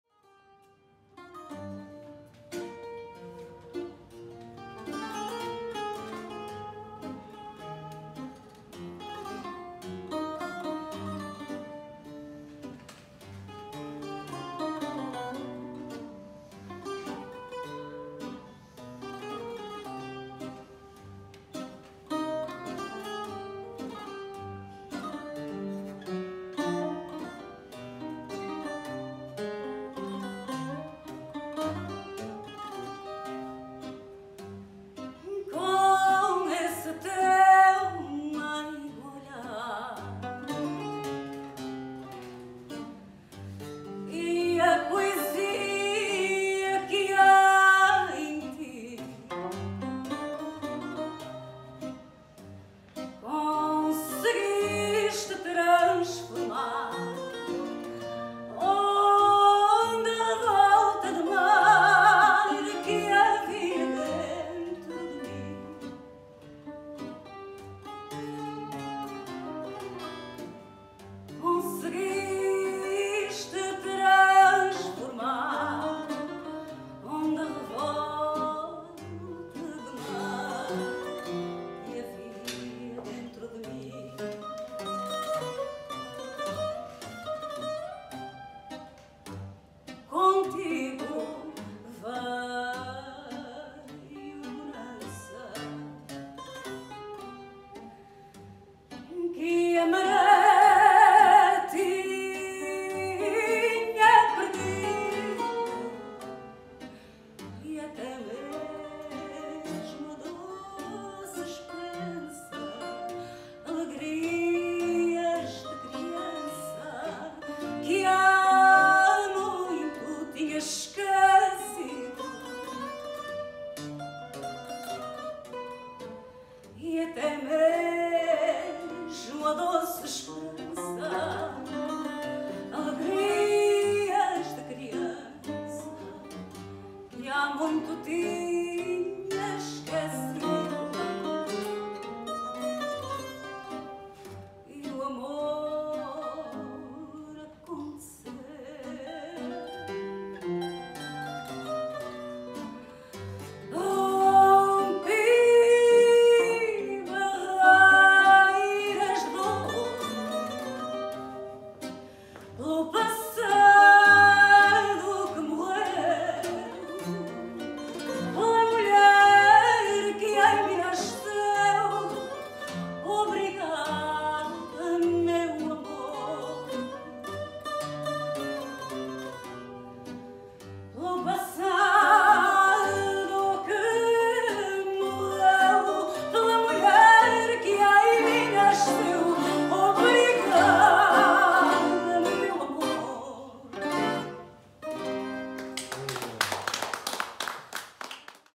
Cátia Oliveira - Na Casa D'Aldeia
Evento de Fados Na Casa D’aldeia
Junqueira Vila do Conde 4 Novembro 2022
A voz de Cátia Oliveira
Acompanhada na Guitarra por Rui Beirão
E na Viola por José Saraiva